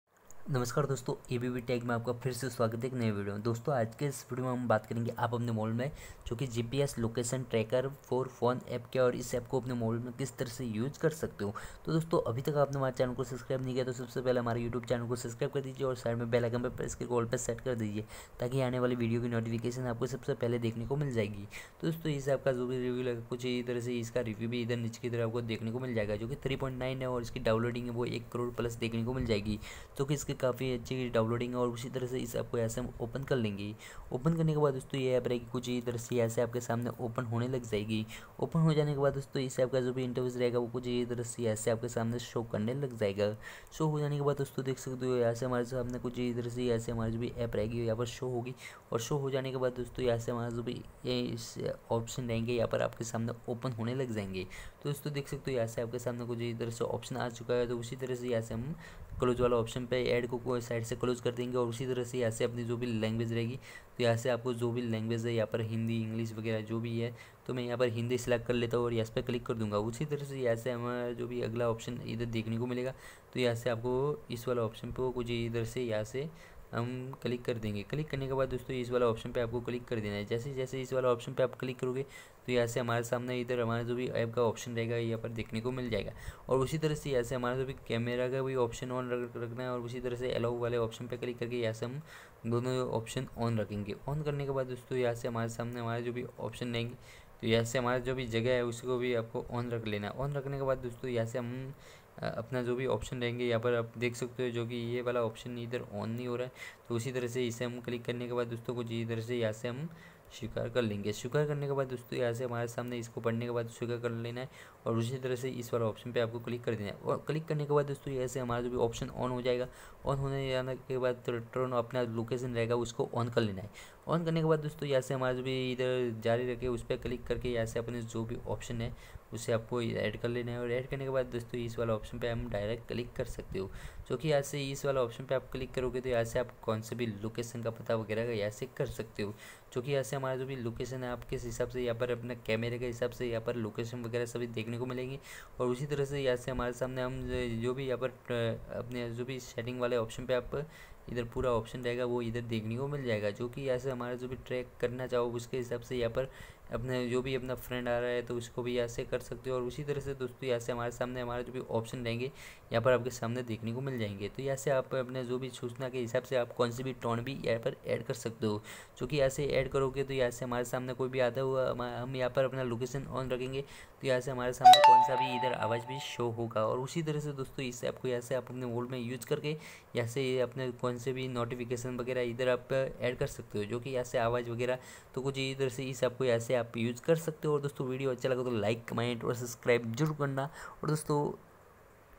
Gps Location Tracker for phone app kaise use kare || How To Use gps Location Tracker for phone app
1, gps Location Tracker for phone app kya hai
2, gps Location Tracker for phone app kaise chalaye
3, gps Location Tracker for phone app kaise use karte hai